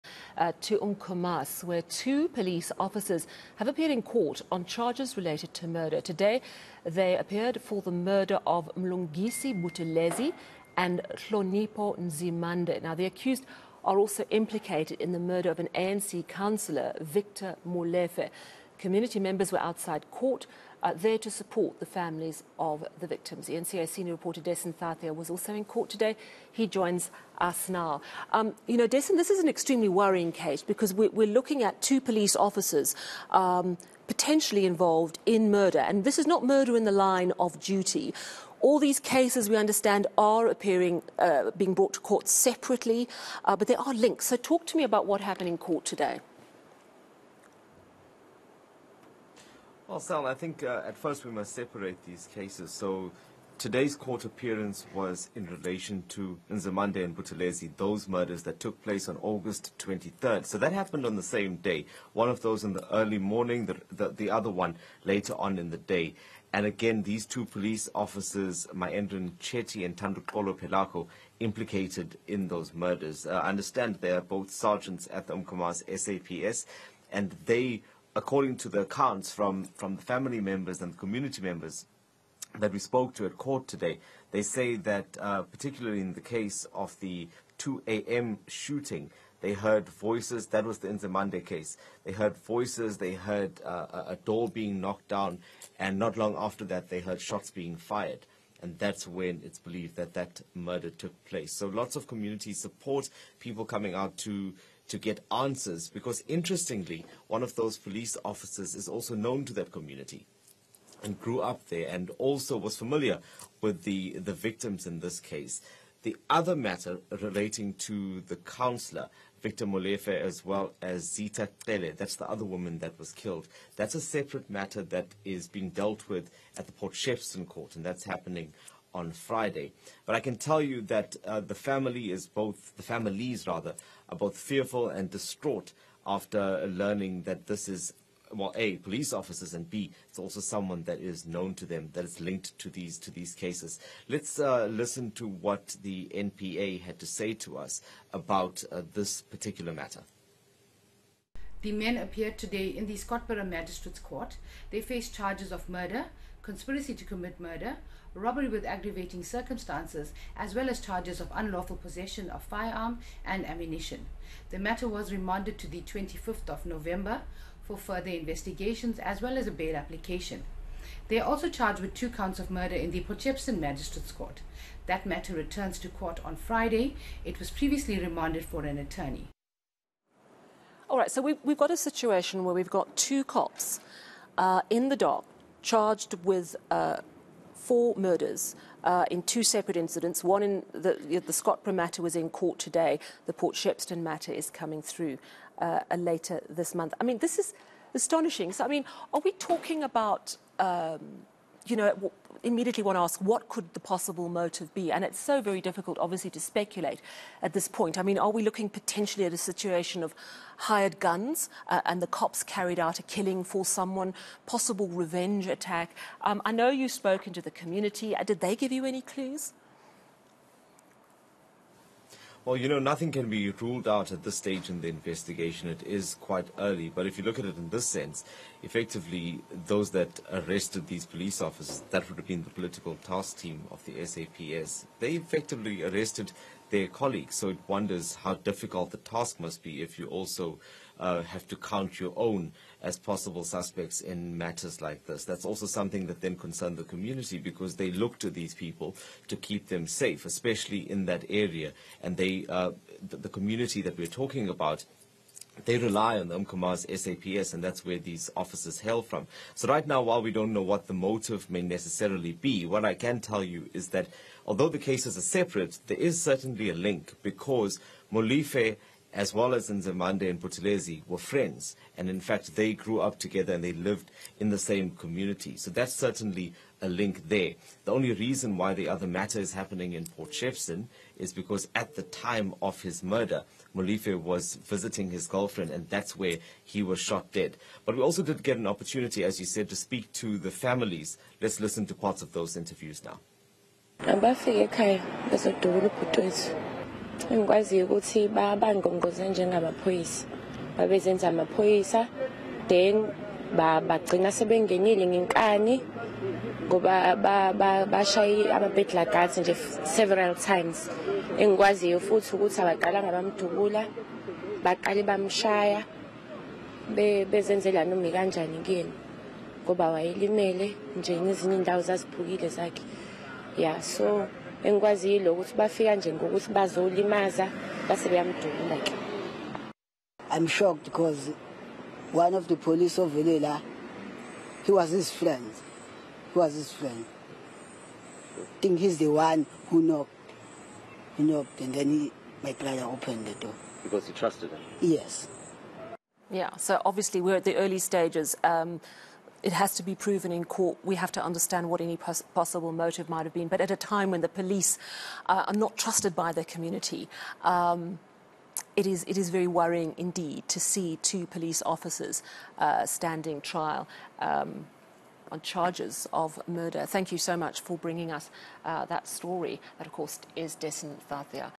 Officers in court for murder
Two police officers have appeared in court on charges related to murder. eNCA's Dasen Thathiah has more details.